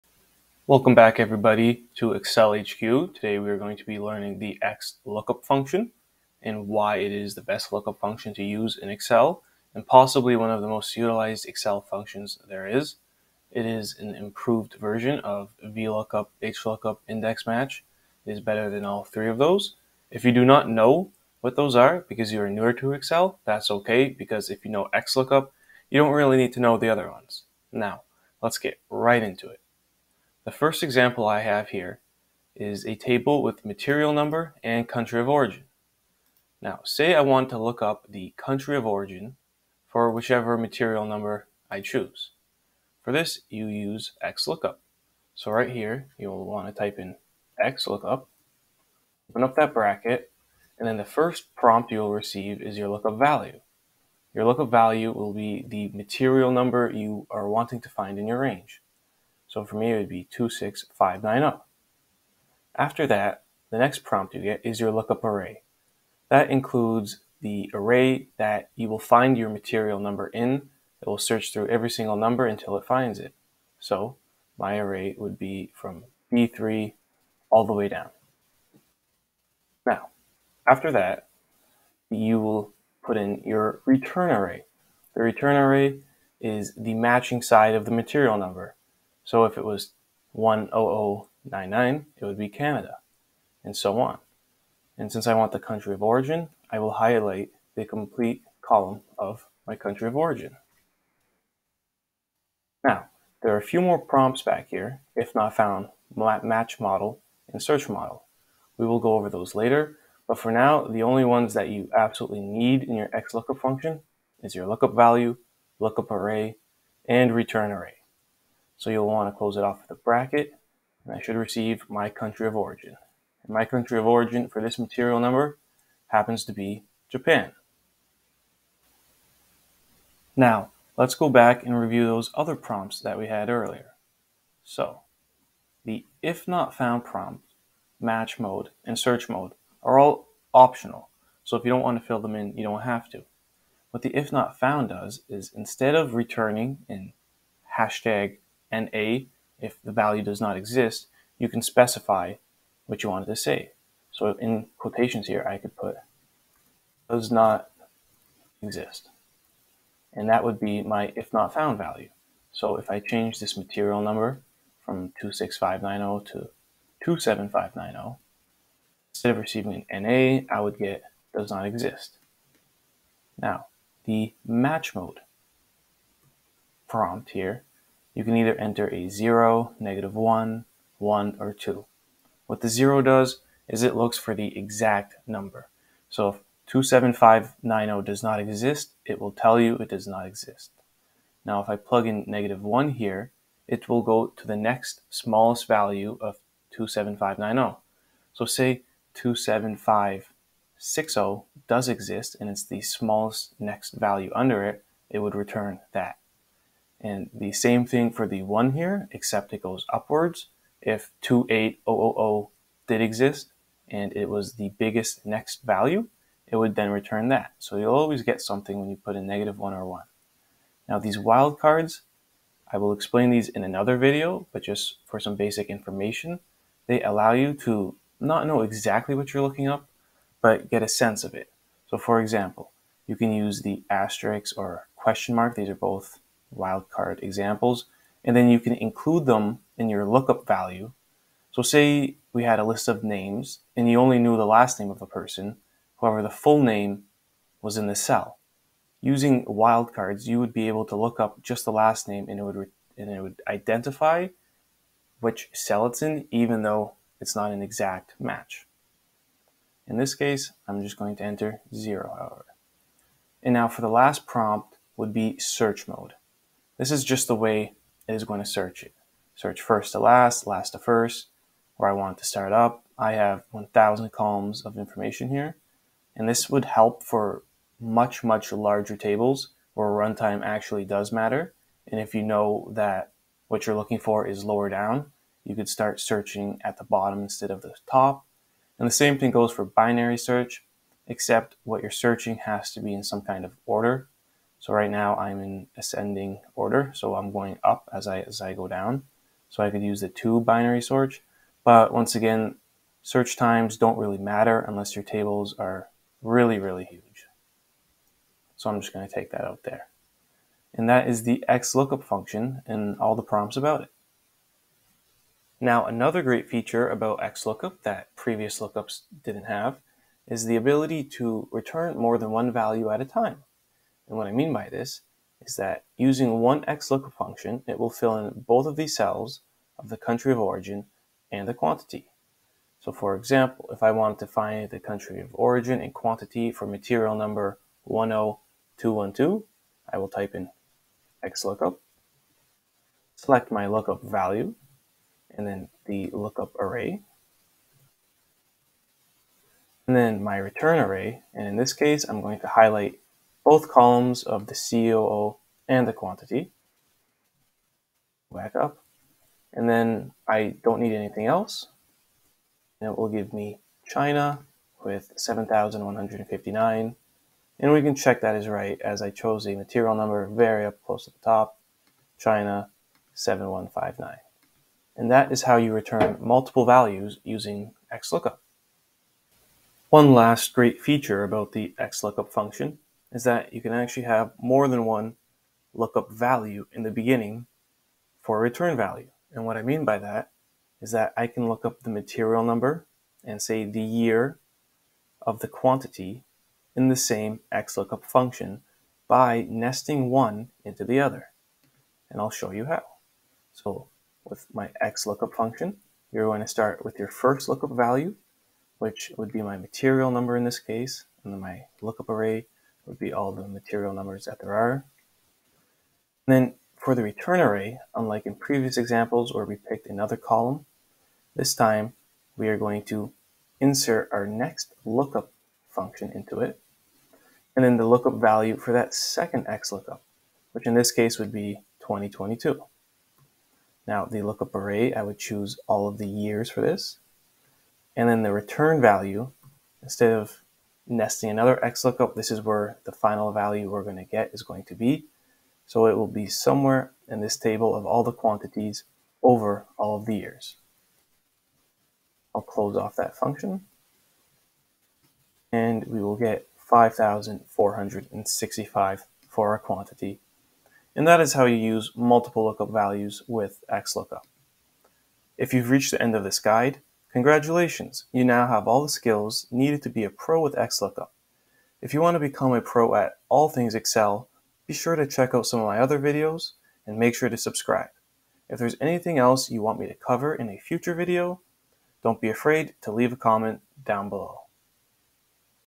XLOOKUP in Excel: The Best Lookup Function
Discover how to use the XLOOKUP() function in excel, along with various different ways you can utilize it to help improve your excel efficiency.

Follow along the series to learn every relevant excel function that will boost your productivity in Excel!

Timestamps:
00:00 XLOOKUP: The Must Know Function in Excel
02:15 "if not found" option
02:54 "match mode" option
04:25 "search mode" option
05:15 multiple return values
06:19 nested xlookup